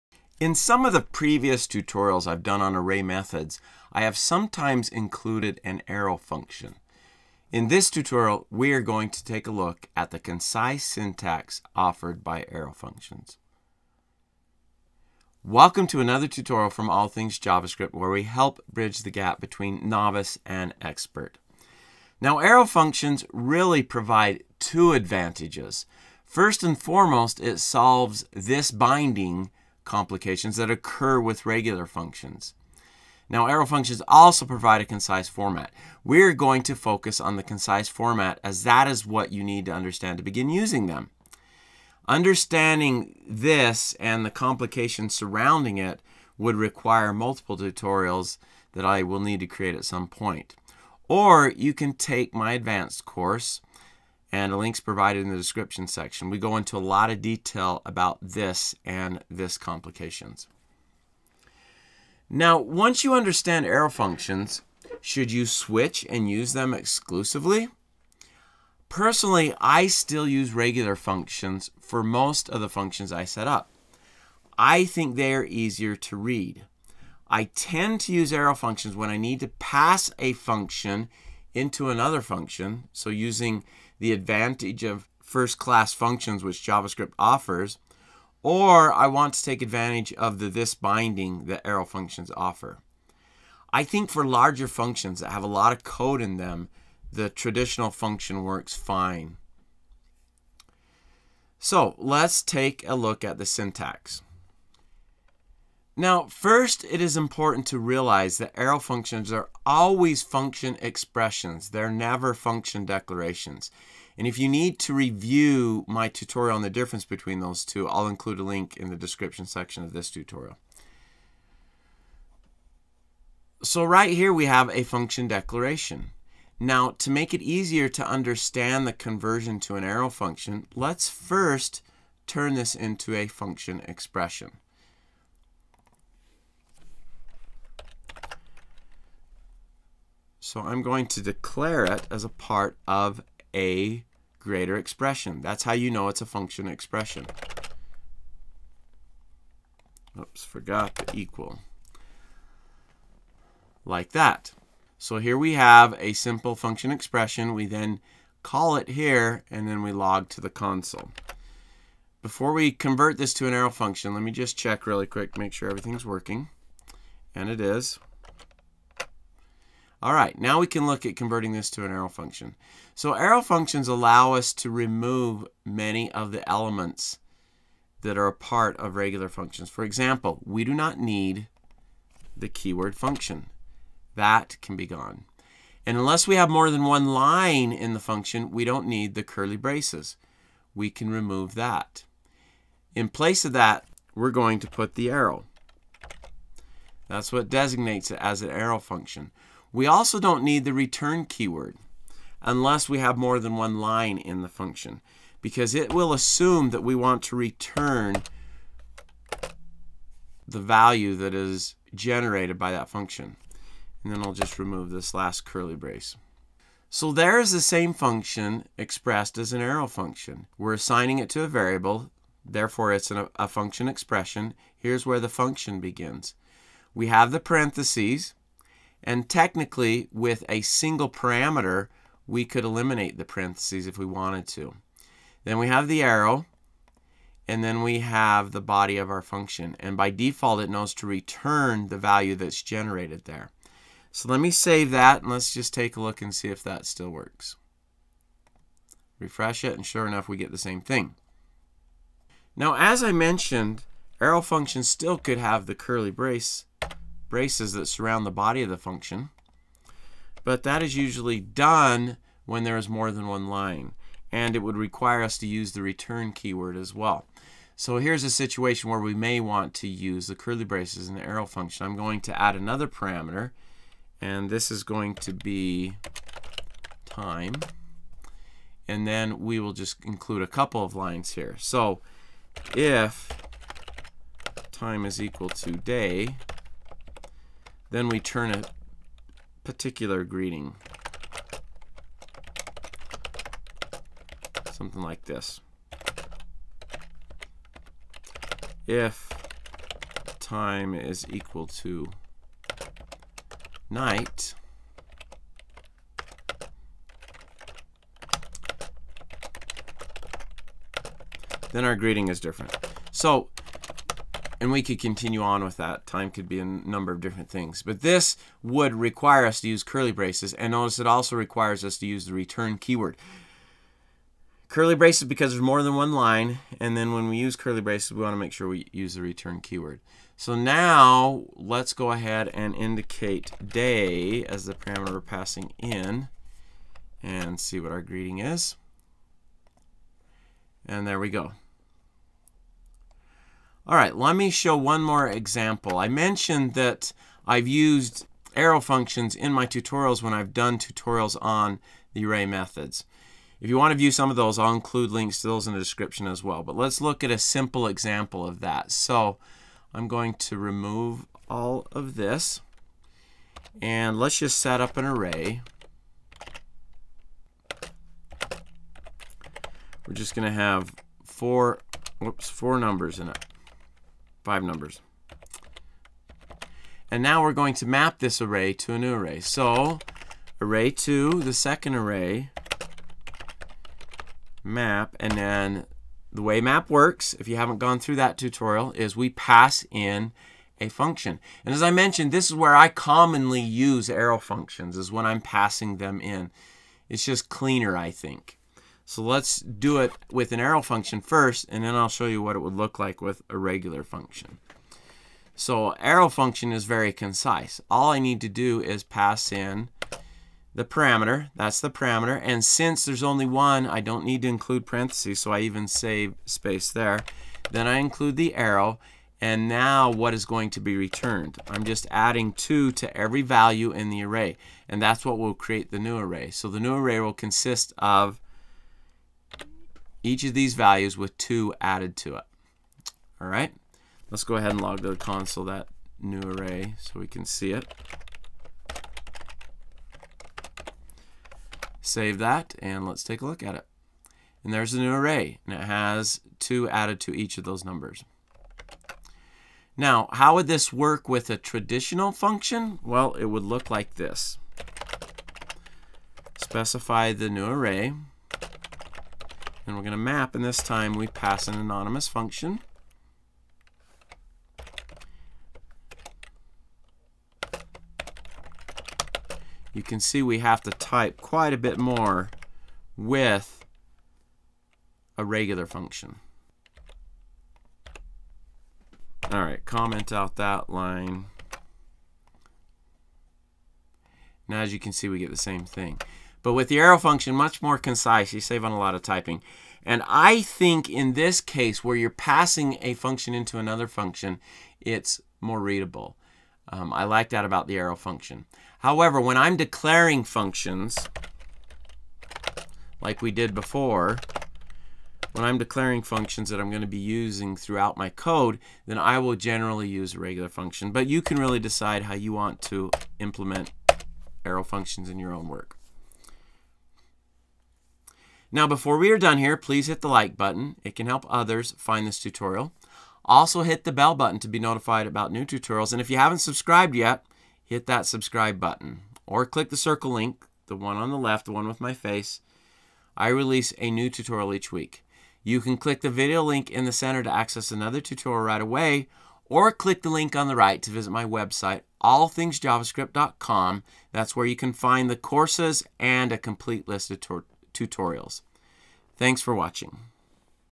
Arrow Functions
In this tutorial we examine arrow functions and the concise syntax provided by this new feature.

Courses offered on Udemy: Getting Started or Advanced Topics at a huge discount: